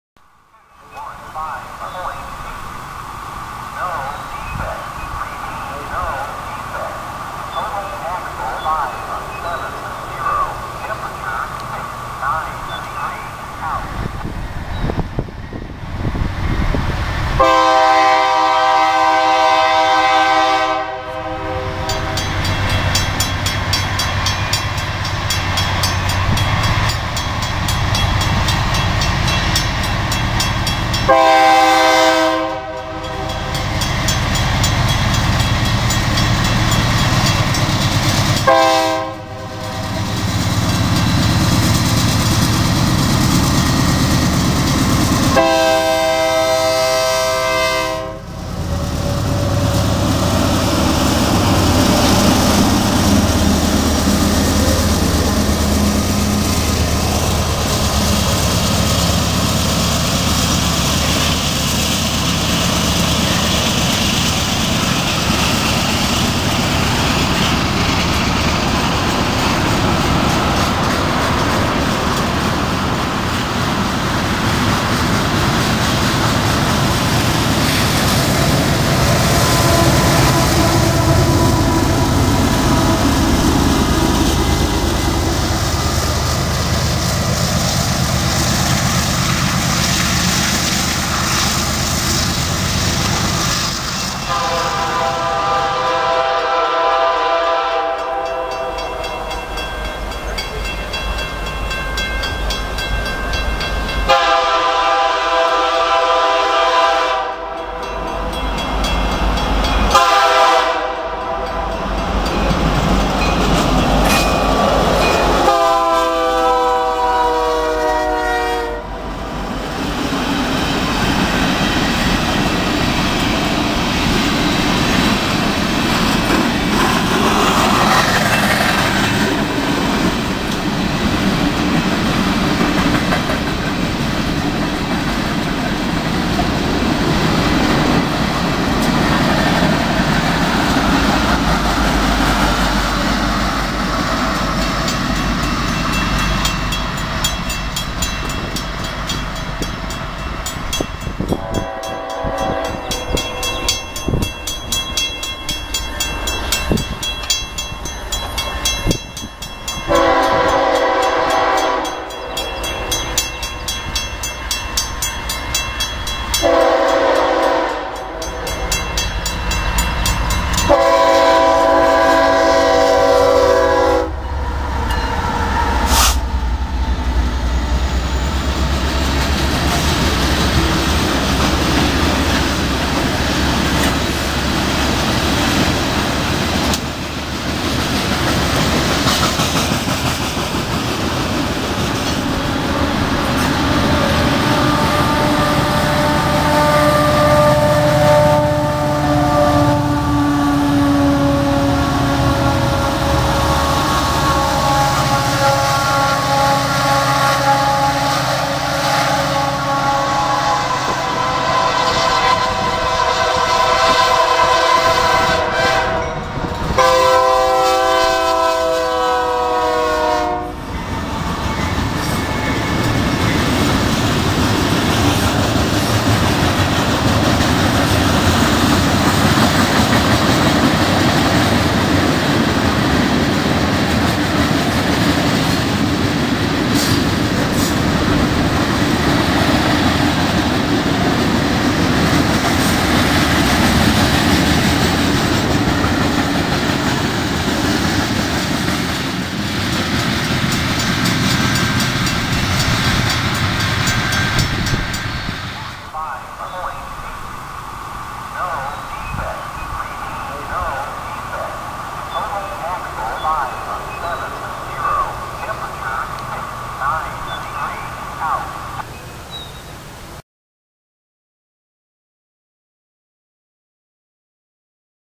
TRAINS HONKING AT INTERSECTION at Turners Station near Springfield, Missouri. Train haul.
Trains running through Turners station intersection. It is located just a few miles east of Springfield, Missouri. Train haul.